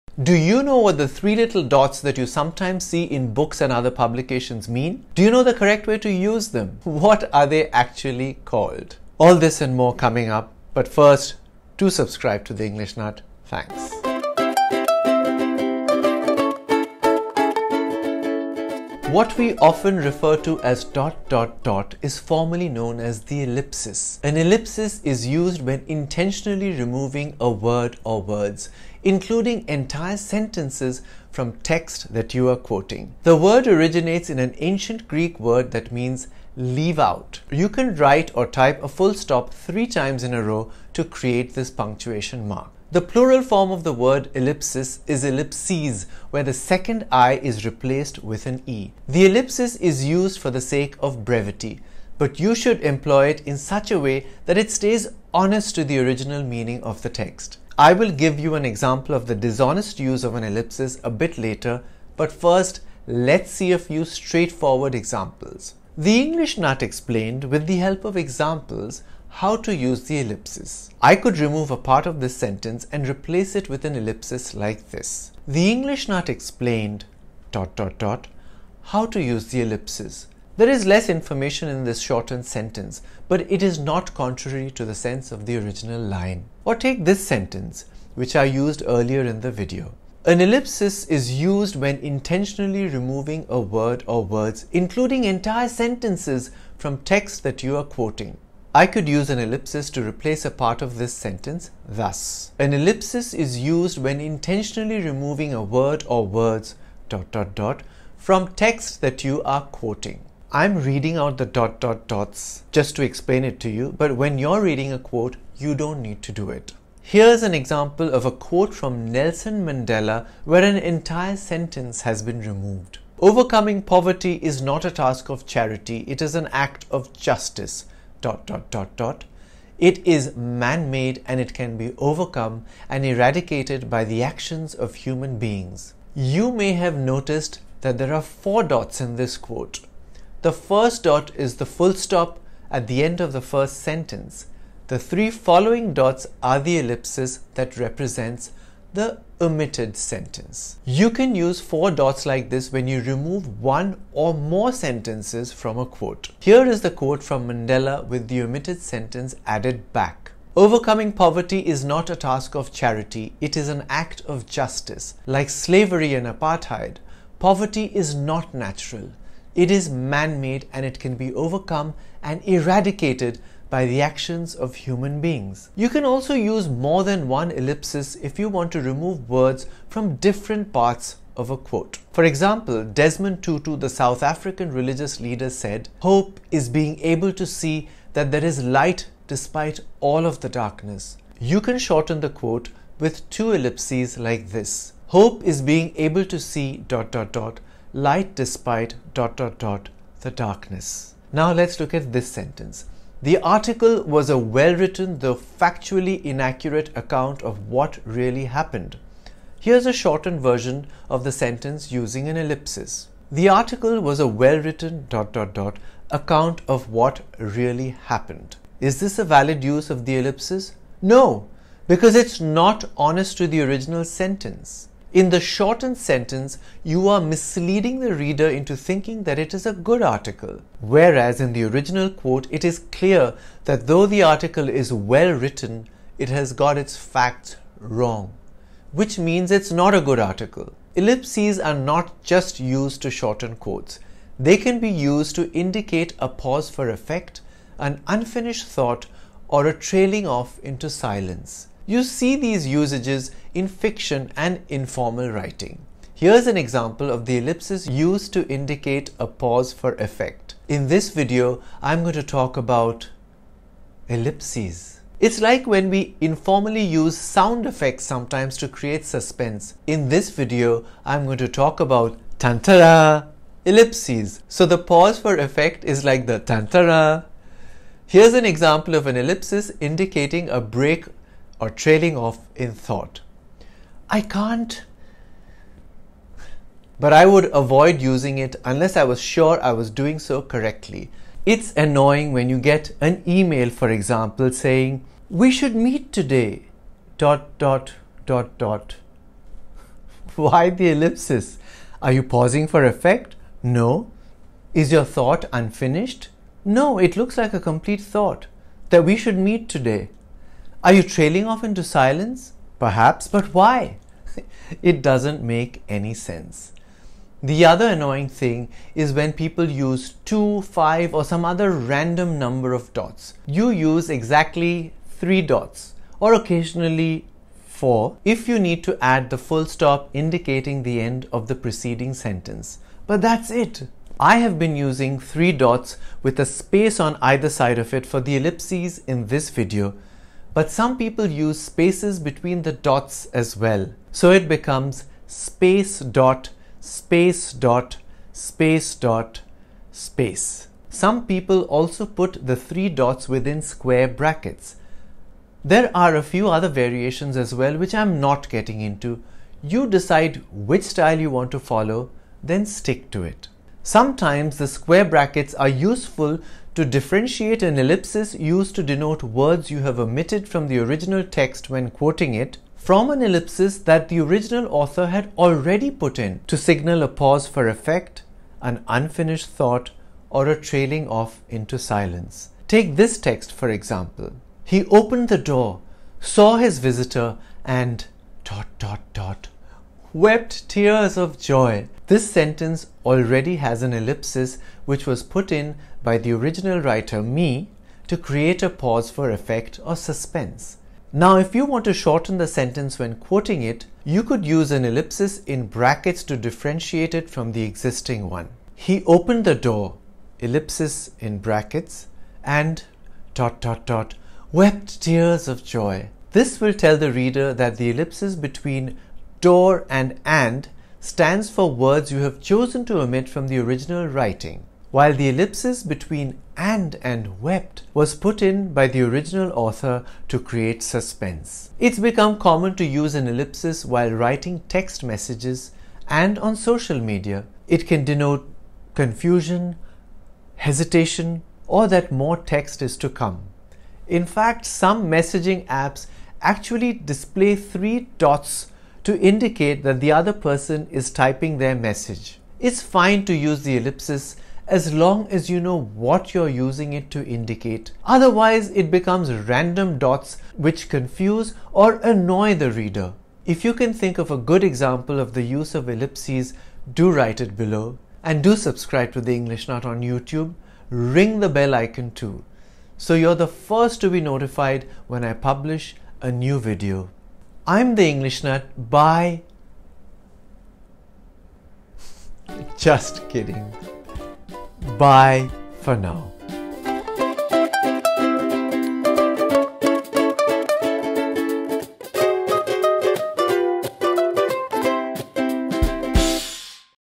DOT-DOT-DOT ... Use the dots right!
Do you know what the three little dots that you sometimes see in books and other publications mean? Do you know the correct way to use them? What are they actually called? All this and more coming up. Thanks.

Episode # 110
TITLE: Dot-dot-dot. How to use it correctly.

What we often refer to as dot-dot-dot is formally known as the ellipsis. Learn the right way to use it. And the correct way to write it--actually there's more than one way to do so.

The ellipsis is used for the sake of brevity. But you should employ it in such a way that it stays honest to the original meaning of the text. I will give you an example of the dishonest use of an ellipsis a bit later, but first we will see a few straightforward examples.

I use quotes of people like Nelson Mandela and Desmond Tutu as examples.

I tell you the correct number of dots to use. And why is specific cases, four dots may be required. Also find out whether you can use more than one ellipsis in a quote.

Ellipses are not just used to shorten quotes. They can be used to indicate a pause for effect, an unfinished thought or a trailing off into silence. You see these usages in fiction and informal writing. I give you examples of these in the video.

Also find out the connection of the ellipsis with the sound effect 'tantara'.

The incorrect or unnecessary use of ellipses is annoying for readers. Learn when not to use them.

Find out about ellipses in square brackets--and if they are useful or not.

It’s become common to use an ellipsis while writing text messages and on social media. It can denote confusion, hesitation or that more text is to come. In fact, some messaging apps actually display three dots to indicate that the other person is typing their message.

It’s fine to use the ellipsis as long as you know what you are using it to indicate. Otherwise it becomes random dots which confuse or annoy the reader.

If you can think of a good example of the use of ellipses, do write it below. So, you’re the first to be notified, when I publish a new video.

I’m The English Nut. Bye …

Just kidding.